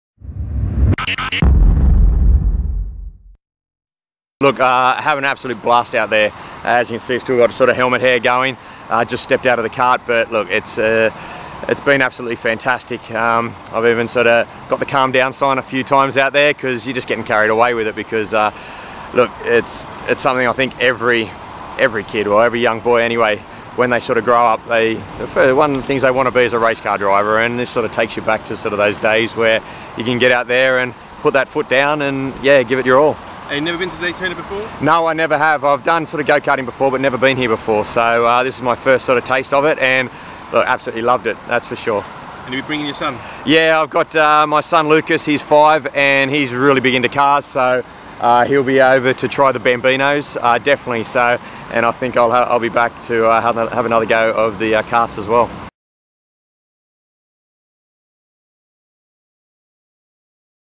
James Pritchard at Daytona
Bedford Blues player James Pritchard visited Daytona Milton Keynes for some flat out fun around the international circuit, what did he think of it?